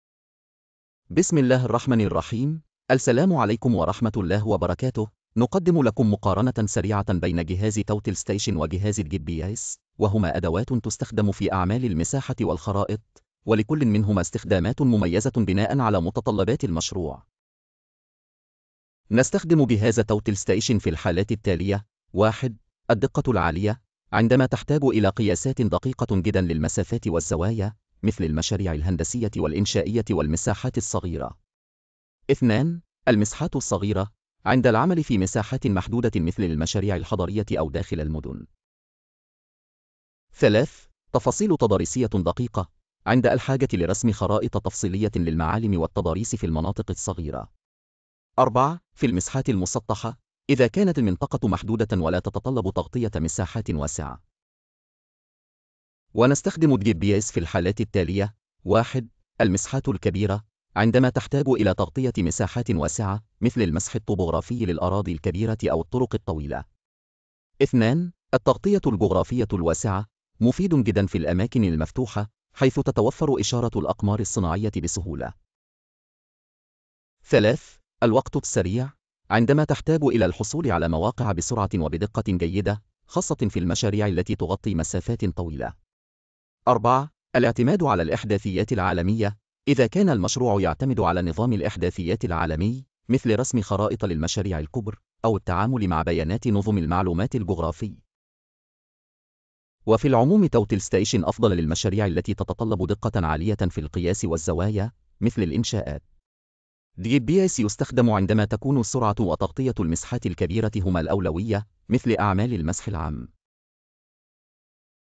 مقارنة سريعة بين الأجهزة المساحية GPS , Total Station
المقالة تقدم مقارنة بين جهاز Total Station وجهاز GPS في أعمال المساحة والخرائط. Total Station يُستخدم في المشاريع التي تتطلب دقة عالية، مثل المساحات الصغيرة والتفاصيل التضاريسية الدقيقة. بينما GPS يُفضل في المساحات الكبيرة والتغطية الجغرافية الواسعة، حيث يُمكن الحصول على المواقع بسرعة. الاختيار يعتمد على متطلبات المشروع، حيث كل جهاز له استخداماته المميزة.